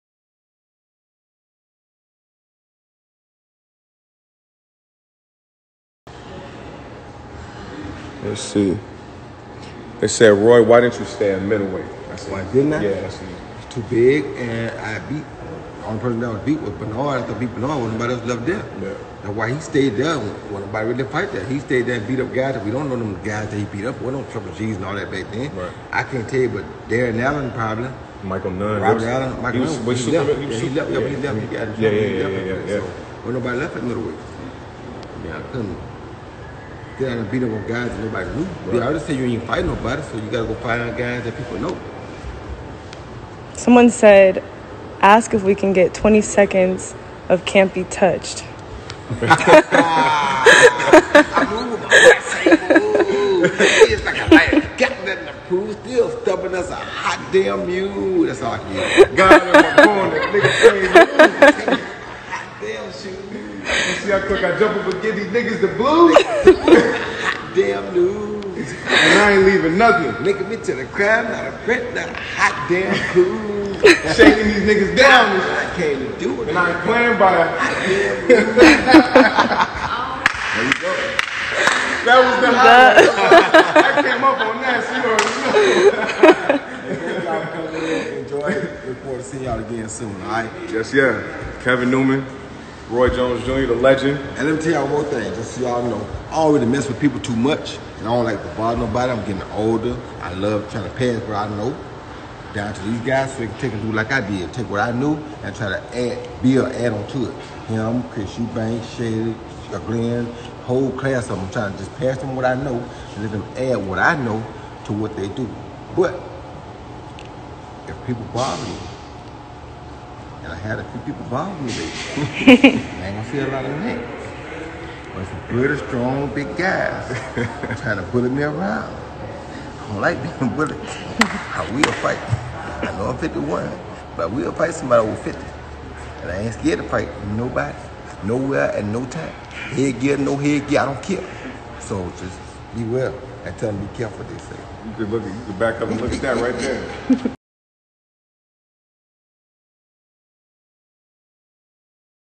ROY JONES JR SENDS STERN WARNING TO MIKE TYSON & EVANDER HOLYFIELD ON POTENTIALLY CALLING HIM OUT!
roy jones jr recently had a chat on social media as he spoke on various things and ended the chat with a stern warning to mike tyson and evander holyfield to be careful what they say and not to call him out because he will fight them